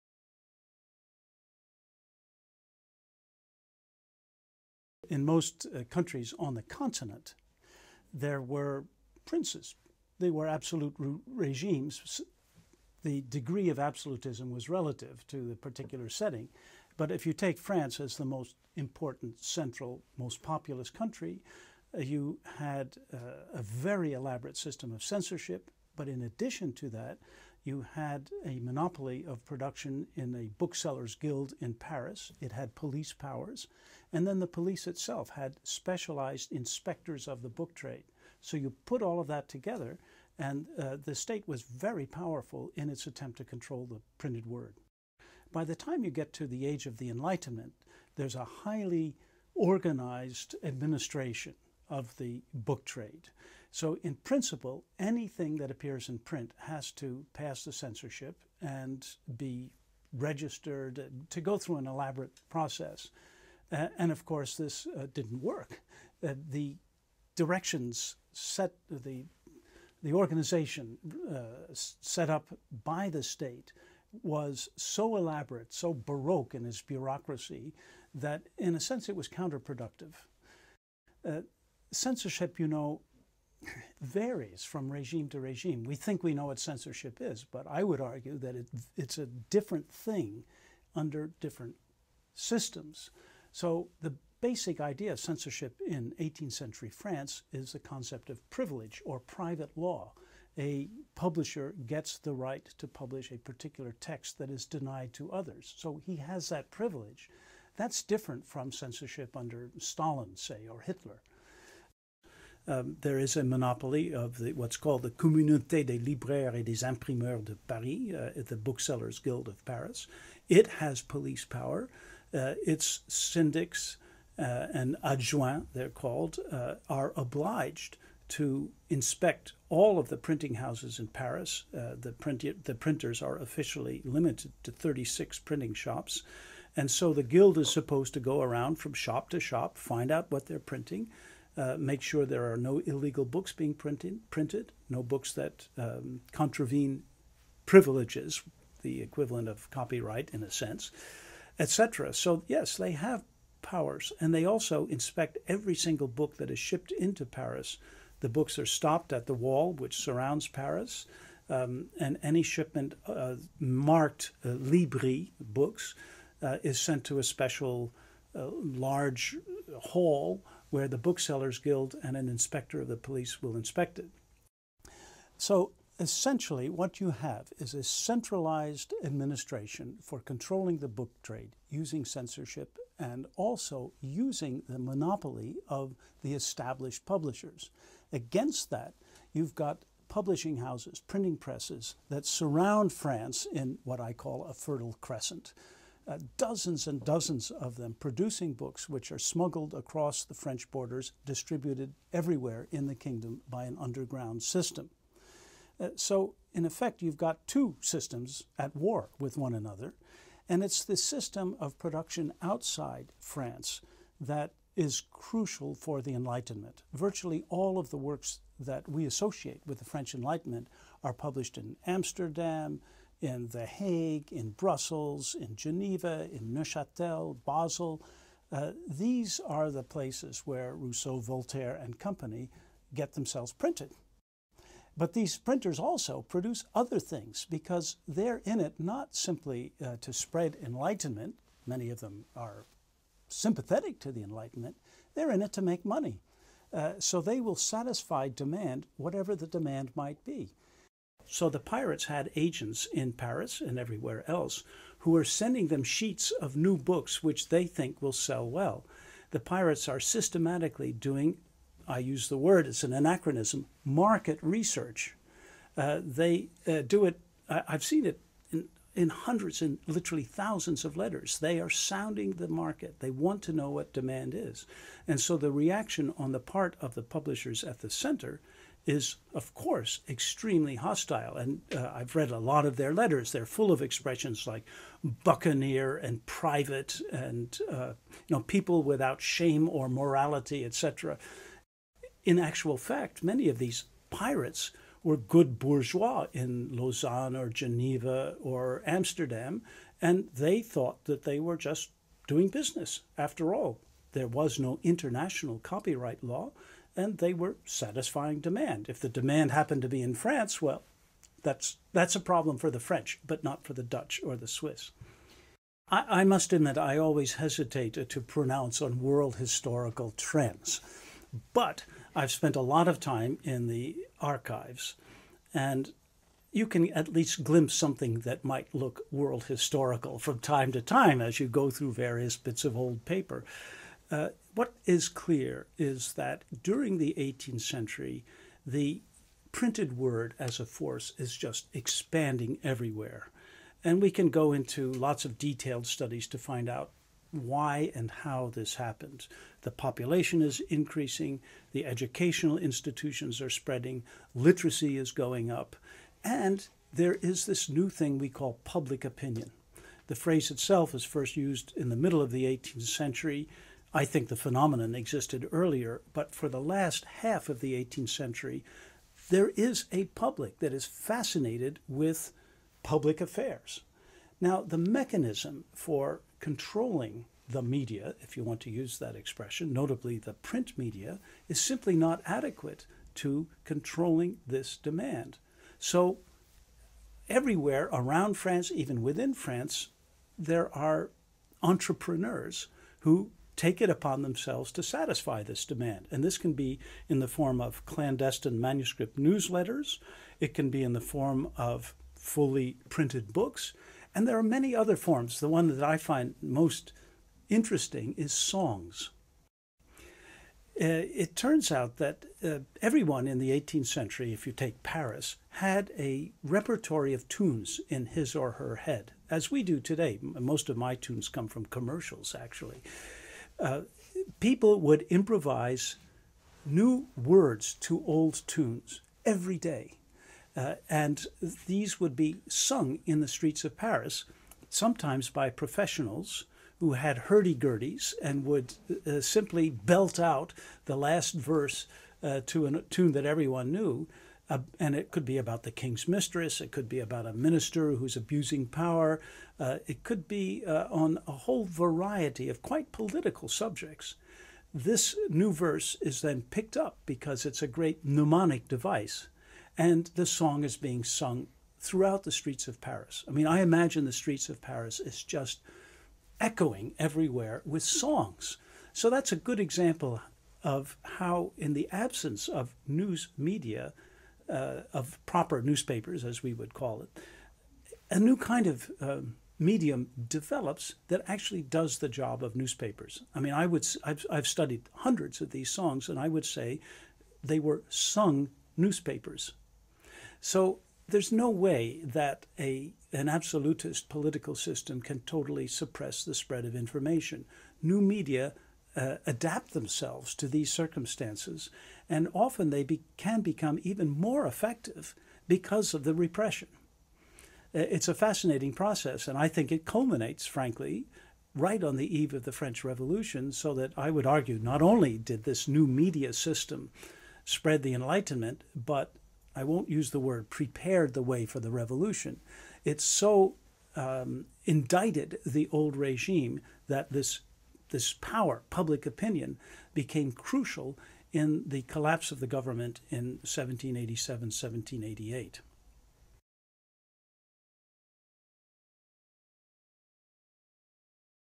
Robert Darnton - Two Information Systems at War, in 18th Century France
Darnton has spent much of his career investigating the system of information control in eighteenth century France prior to the revolution of 1789. This research brought him to Neuchatel in Switzerland, one of the important centers of printing of the period, where materials forbidden in France were published to be later smuggled into the country and distributed through sophisticated networks.